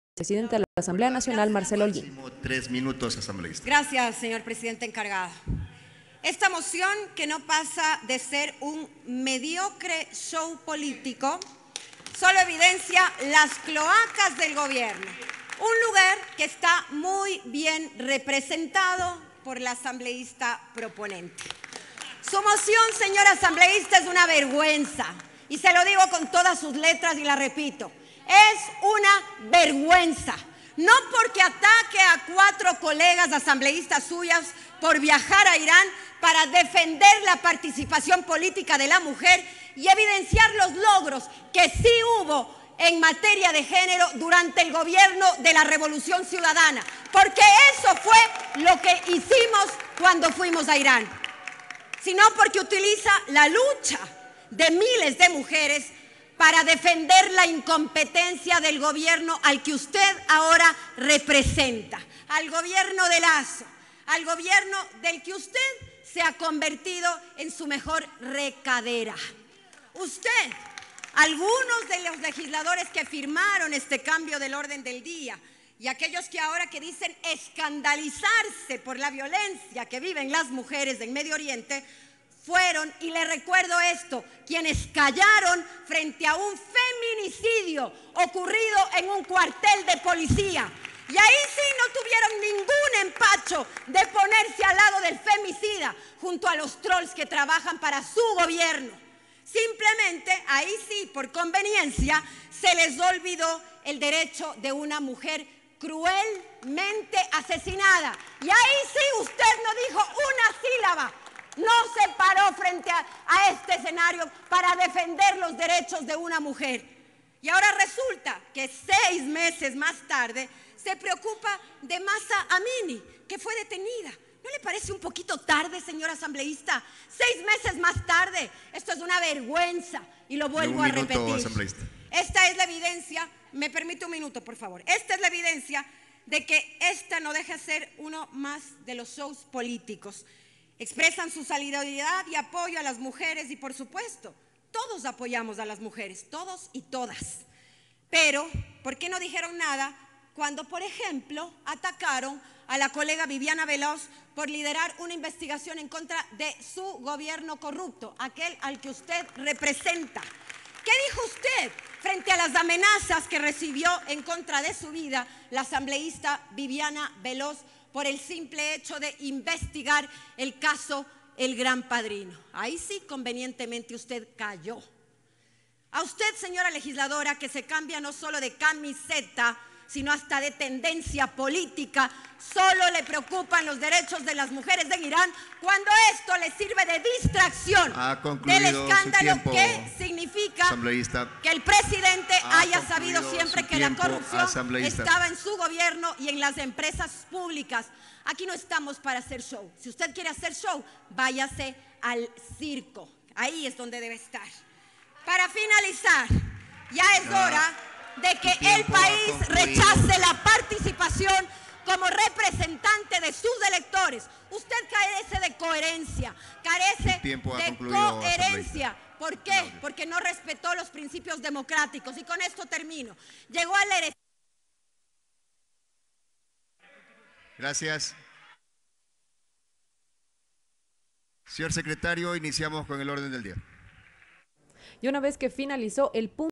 Asambleísta Marcela Holguín - Sesión 853 - Punto de información
14 de marzo de 2023
PUNTO DE INFORMACIÓN | considera que la solicitud de cambio de orden del día "es un show político". Cuestiona que ahora quieran defender los derechos de las mujeres cuando fueron quienes callaron ante un femicidio ocurrido en una Escuela de Policía.